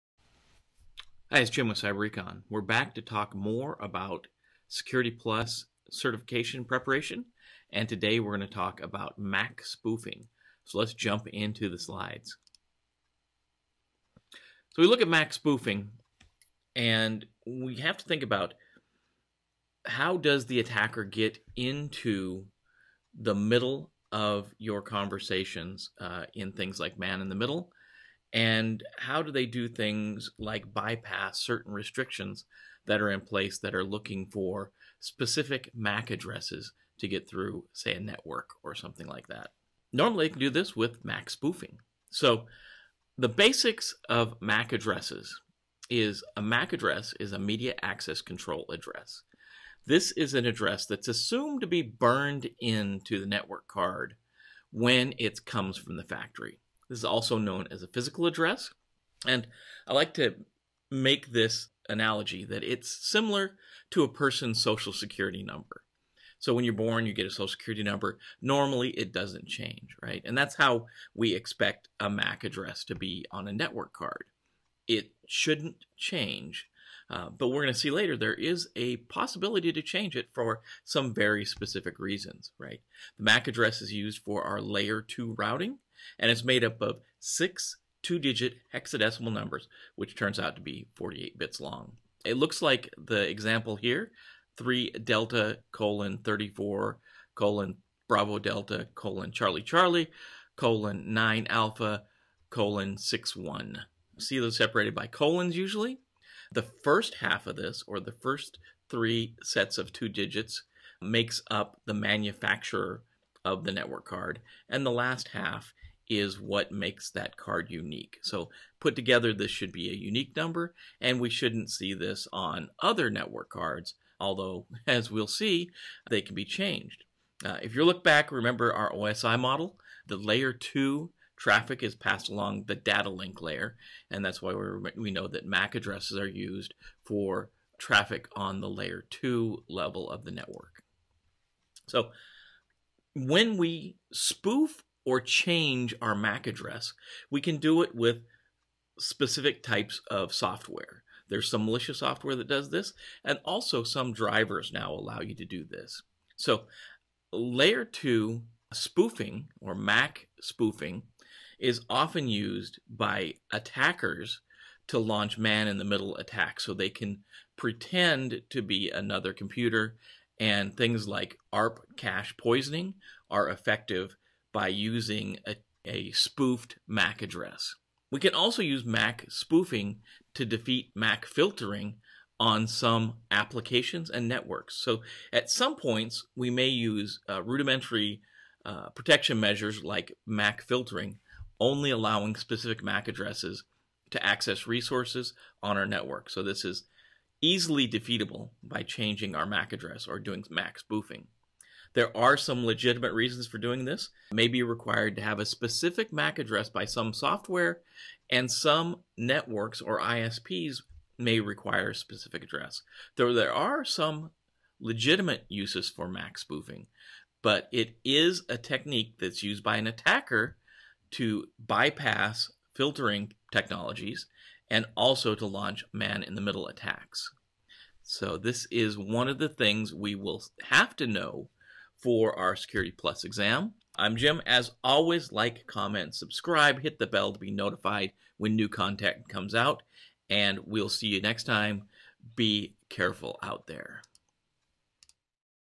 Security + 1.2 MAC Spoofing
Mac spoofing can be a problem for you and your users. In this video will be discussing how to prevent these attacks and defending against them.

Join us as we take the month of April 2020 to prepare you for the Security+ exam in these videos were going to break down everything you're going to need to know to get your CompTIA certification.